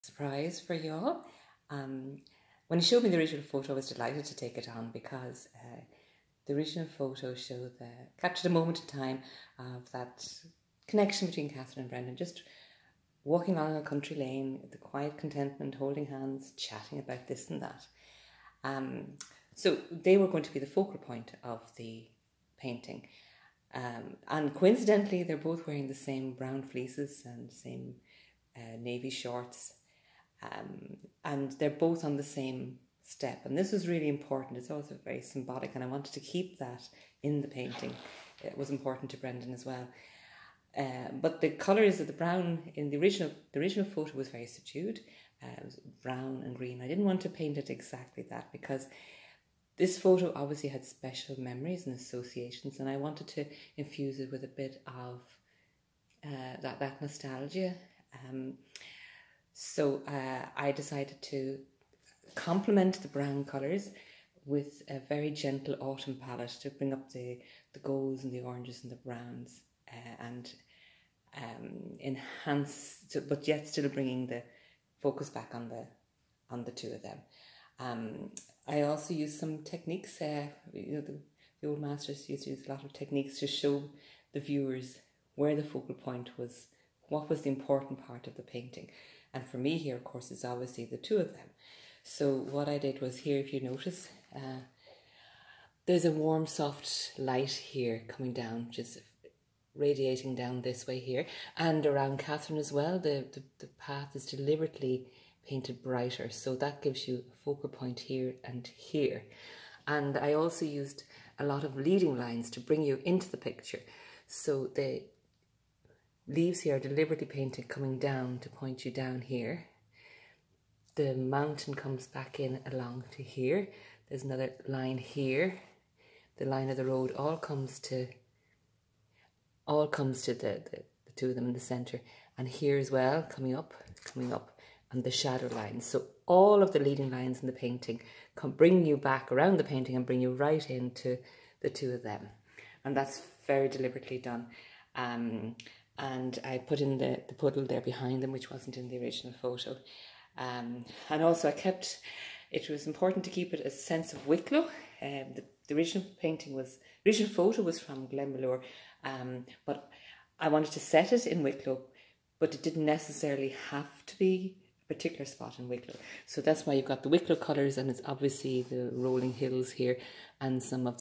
"In Step Together", by Margaret Farrell Artist
One of my favourite commissions of Dad Brendan and his daughter.